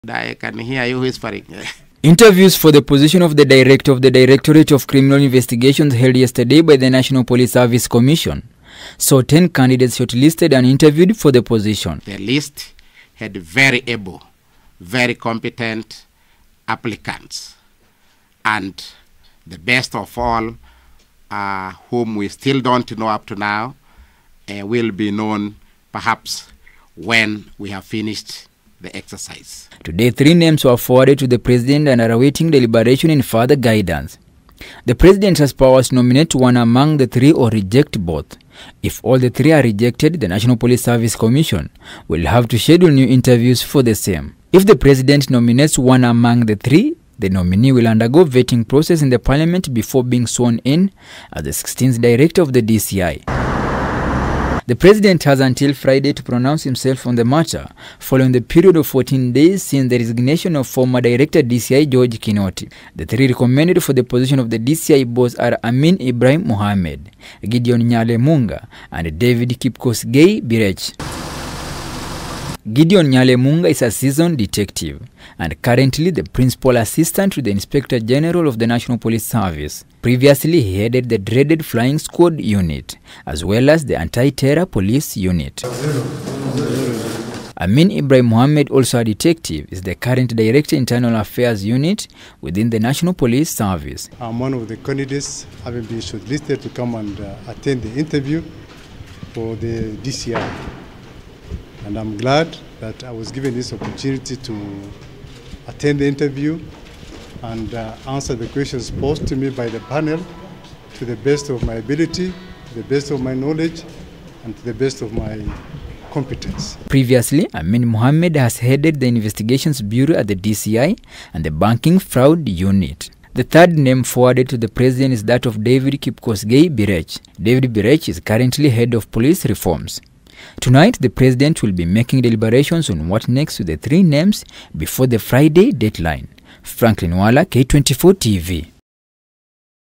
The President has until Friday to make pronouncement of the new DCI director
Three names have been forwarded to the president for consideration for the position of director, directorate of criminal investigations. The president has until Friday to make a pronouncement on the matter or recommend one among the three who will undergo the vetting process in parliament before being sworn in as the new DCI director.